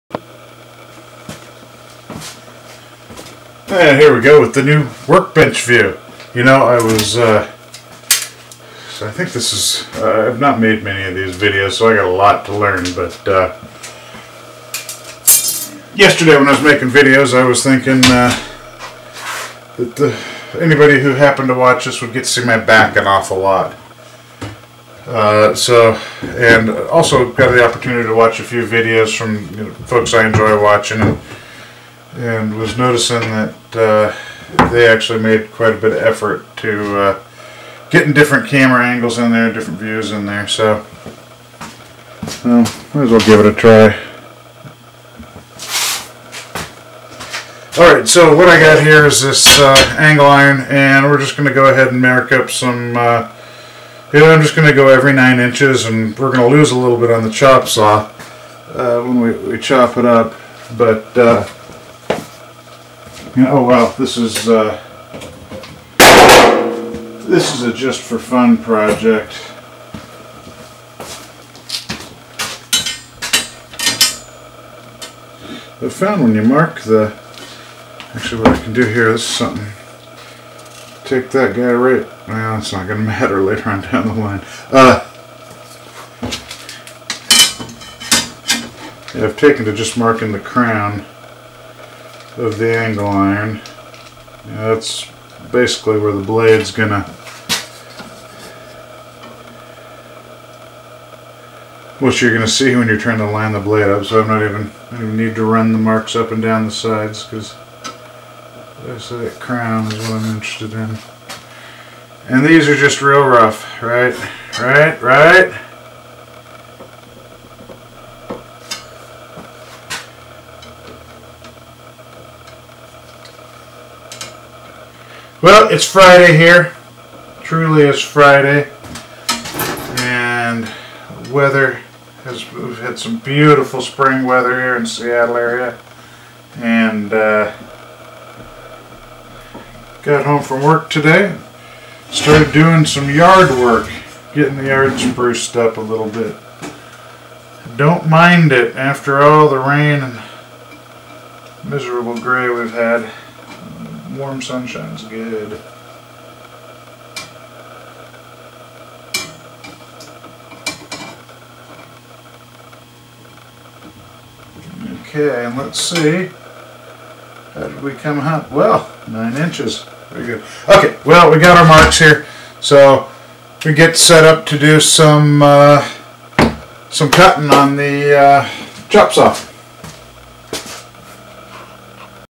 Making a warming rack for the campfire - 3 of 18 (unedited)
Wanted to weld up a rack for warming coffee by the campfire and ended up getting a bit artsy and making a gift for a friend.